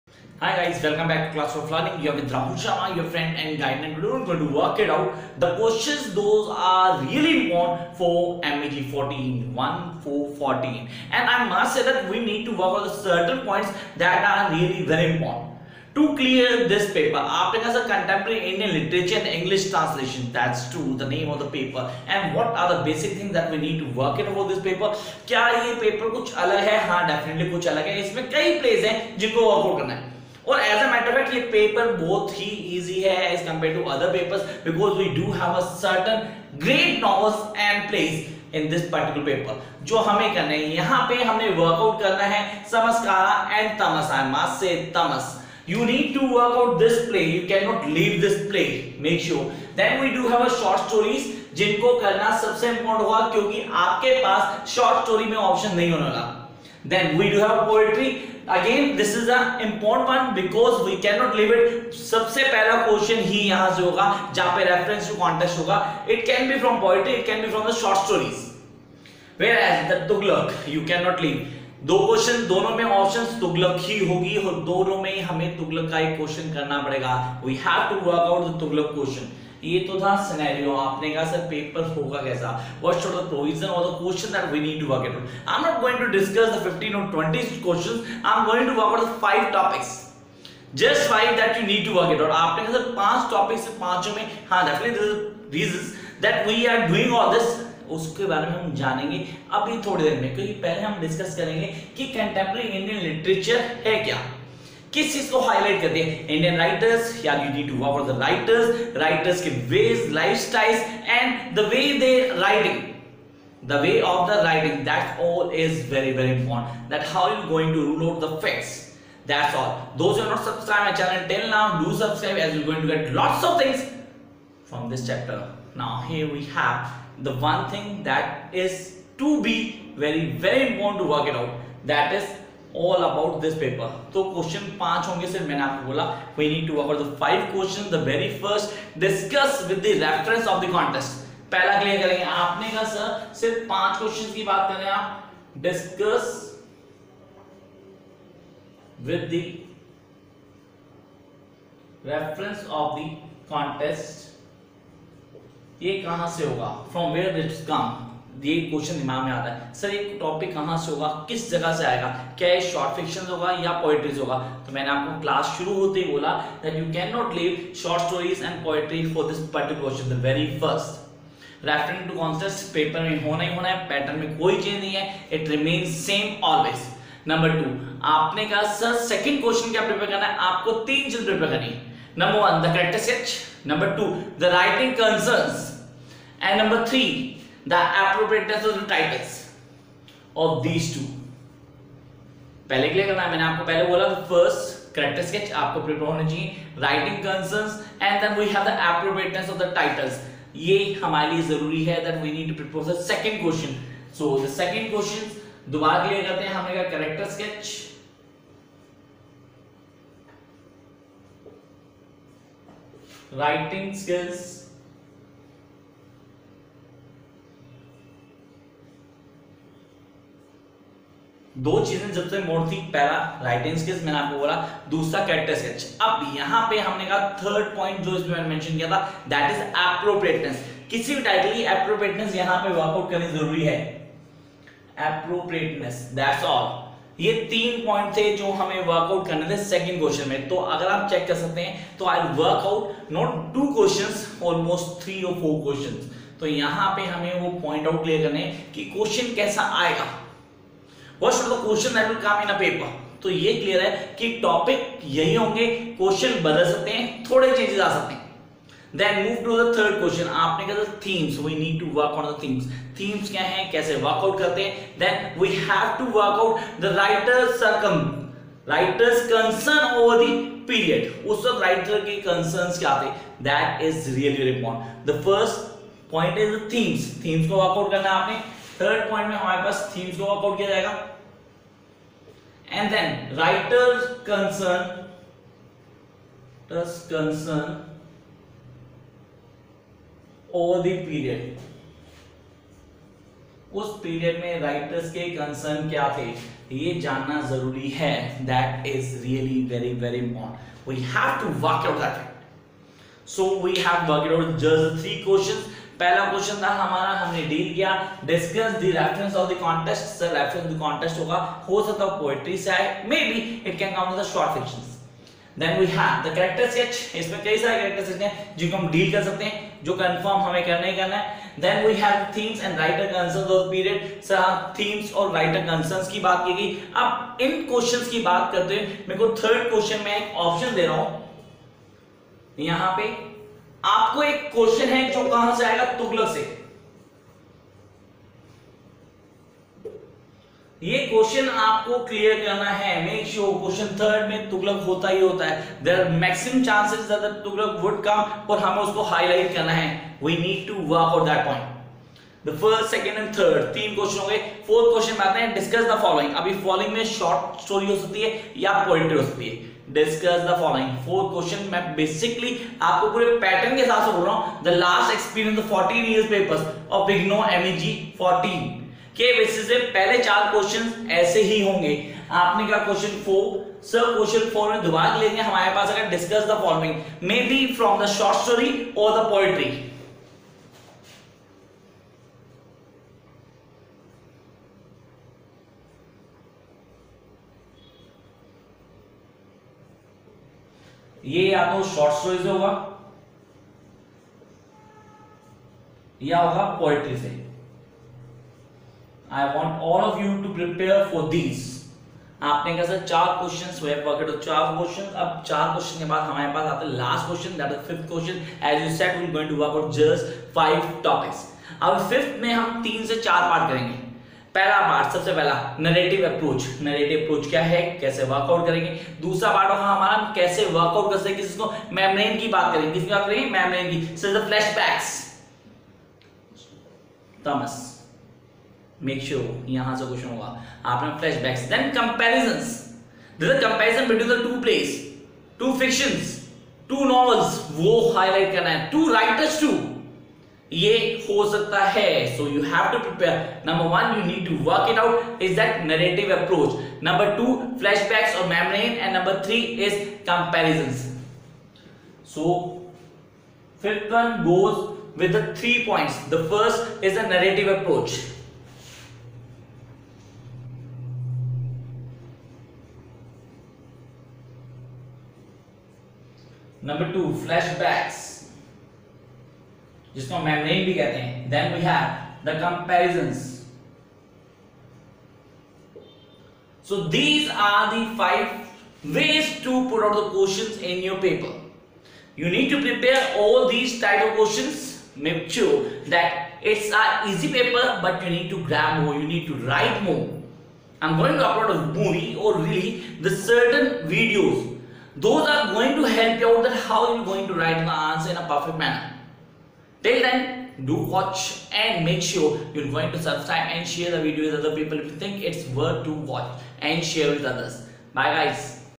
Contemporary Indian literature in English Translations Meg 14
Meg 14 IGNOU
Important questions for Meg 14 June 2018
By RAHUL SHARMA